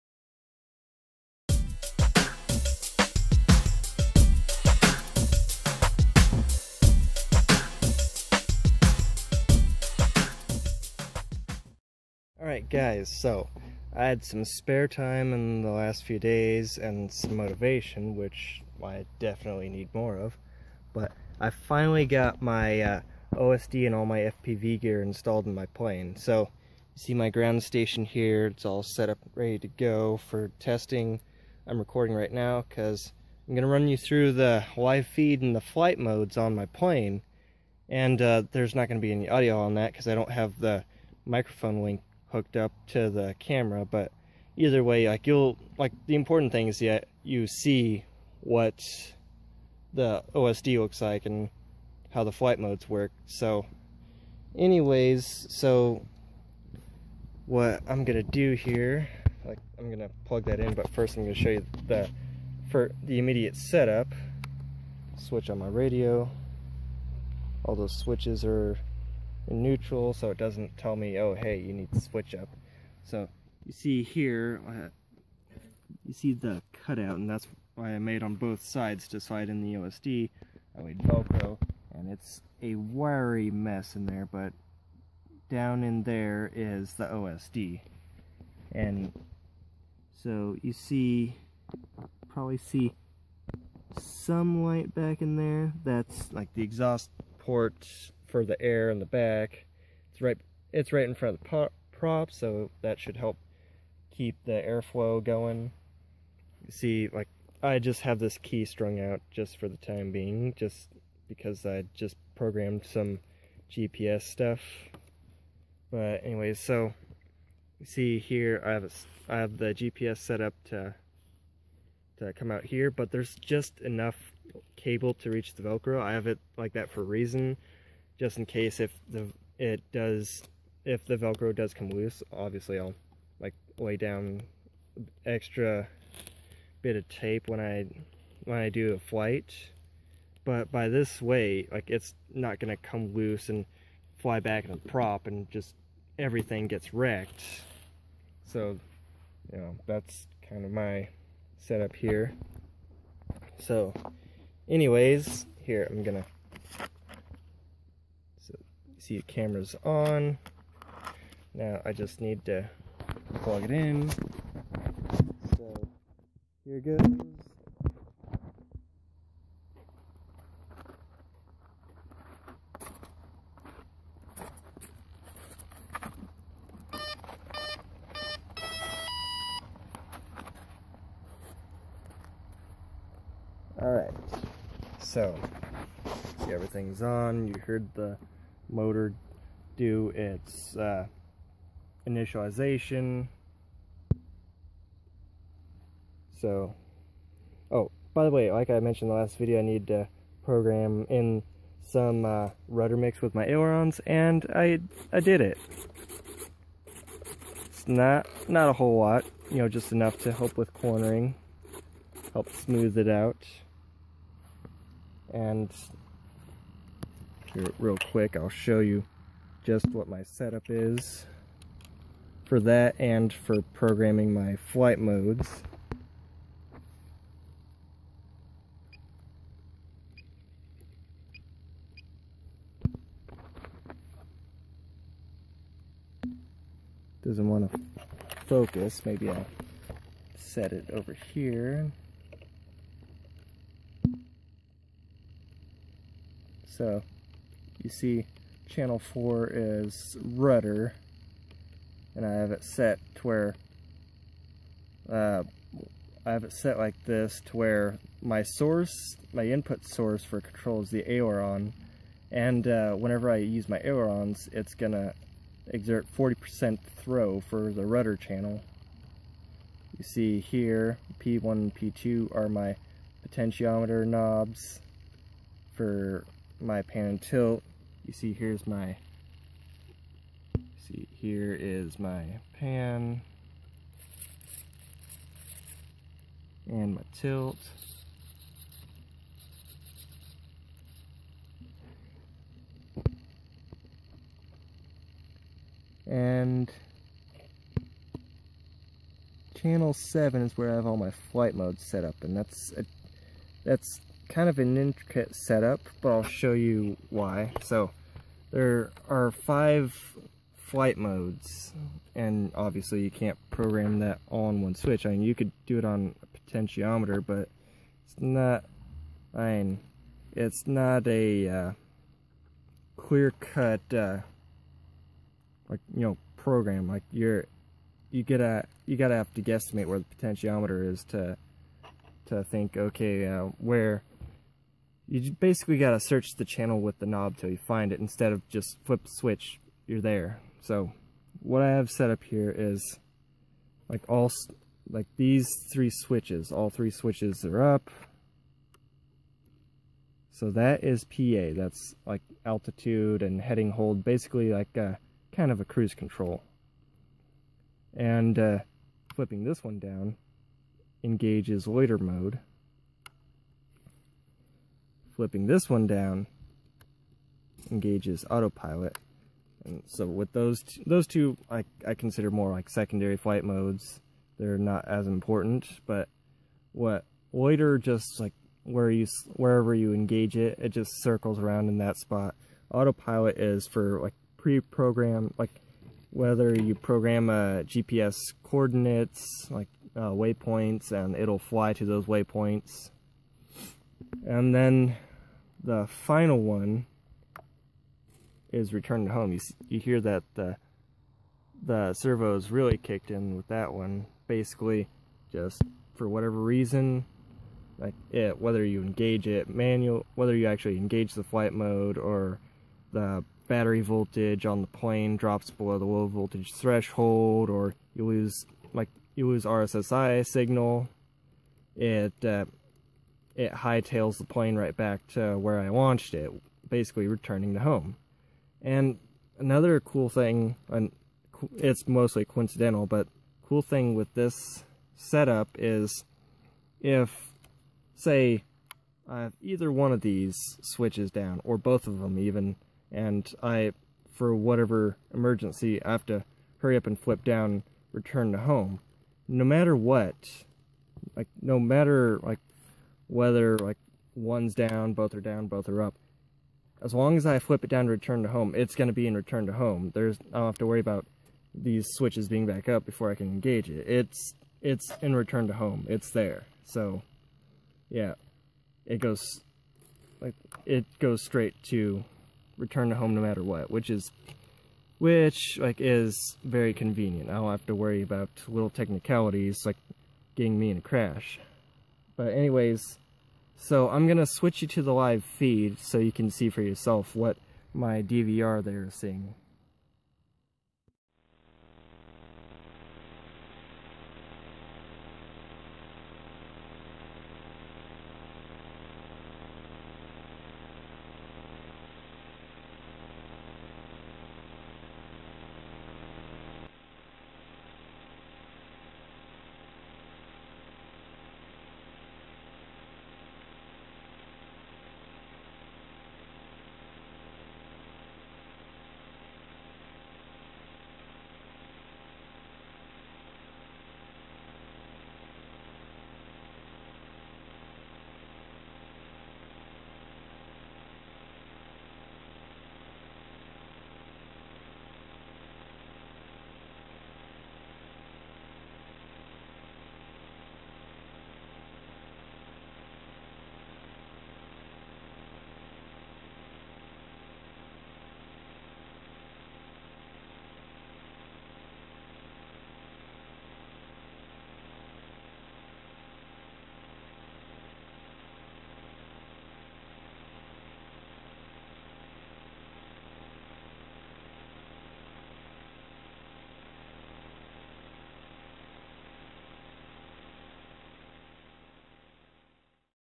FPV gear is installed - BENCH TEST
I just need to get the air tube set up and it'll be ready for FPV. Of course, still need to test fly it.